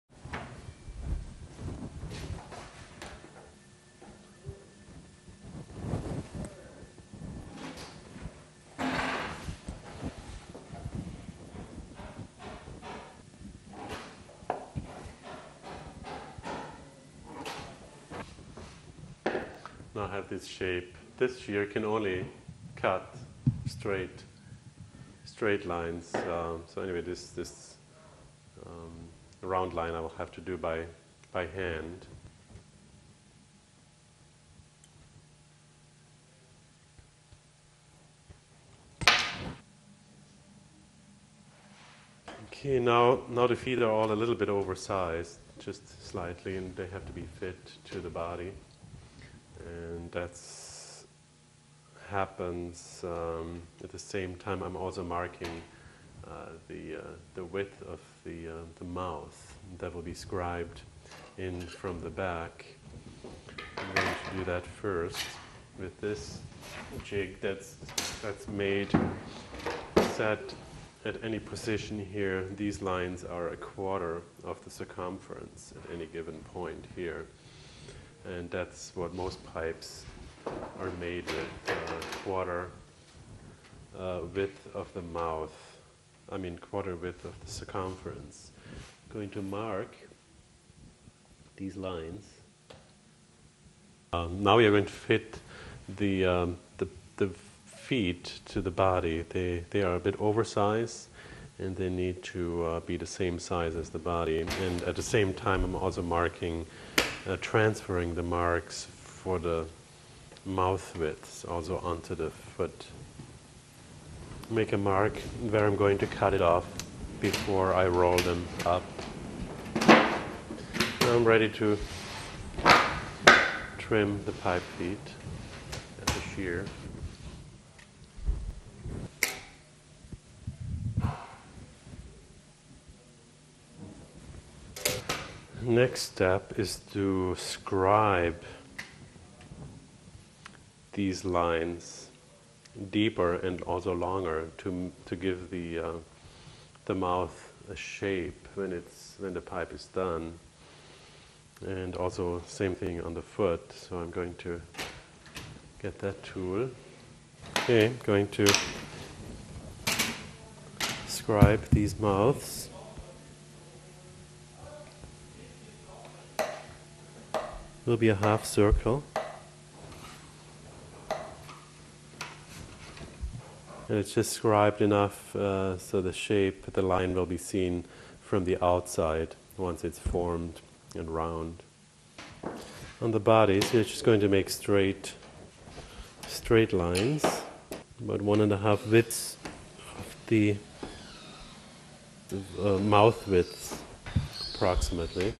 Pipe Making Part 4: Cutting, Marking and Scribing
This excerpt is taken from a documentary film on the construction of Martin Pasi's Opus 14, It shows the cutting, marking and scribing of the metal for an organ pipe. This segment was filmed by Mike Cousino in the Pasi Organ Builders shop in Roy, Washington. Part 4 of 6.